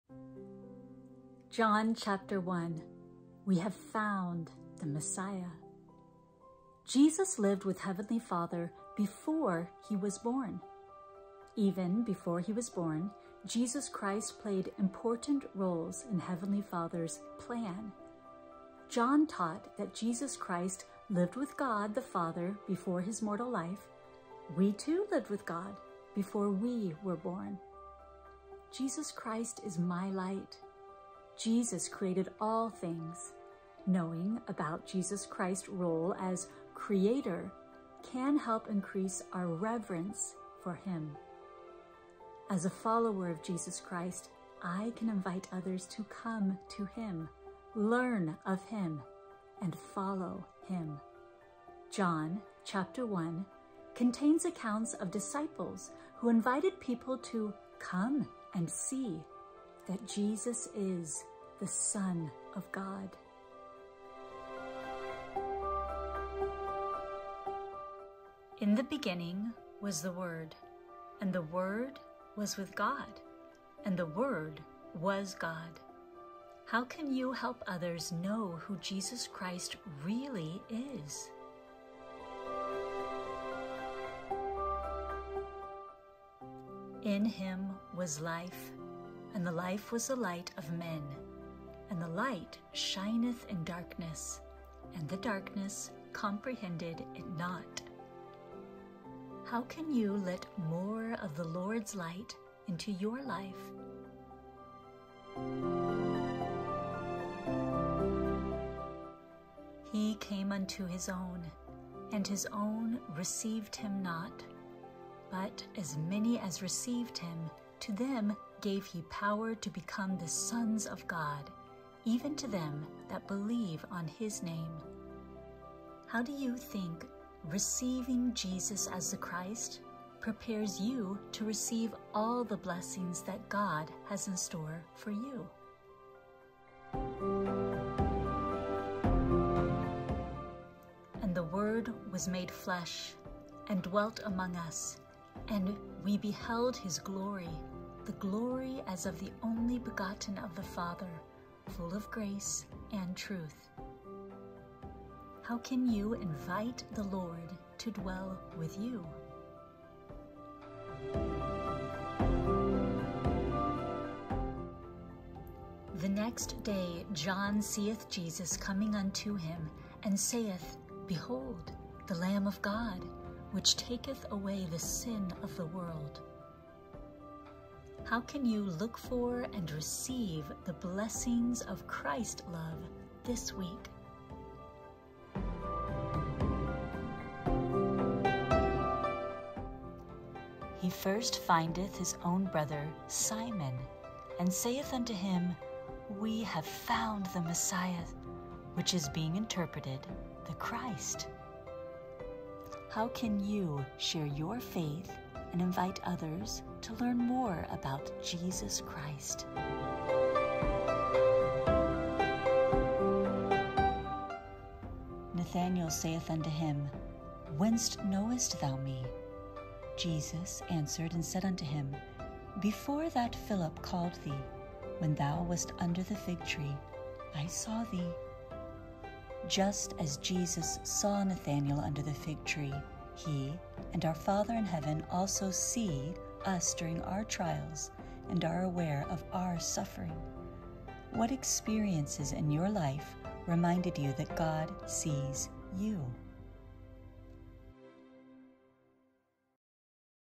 NT 3.) John 1: WE HAVE FOUND THE MESSIAH * COME, FOLLOW ME with Joy
COME FOLLOW ME with Joy: New Testament Bible

3.) John 1: WE HAVE FOUND THE MESSIAH

Jesus lived with Heavenly Father before He was born. Even before He was born, Jesus Christ played important roles in Heavenly Father’s plan. John taught that Jesus Christ lived with God the Father before His mortal life. We too lived with God before we were born.

Jesus Christ is my light. Jesus created all things. Knowing about Jesus Christ’s role as Creator can help increase our reverence for Him.

As a follower of Jesus Christ, I can invite others to come to Jesus Christ, learn of Him, and follow Him. John 1 contains accounts of disciples who invited people to “come and see” that Jesus is the Son of God.
~COME, FOLLOW ME with Joy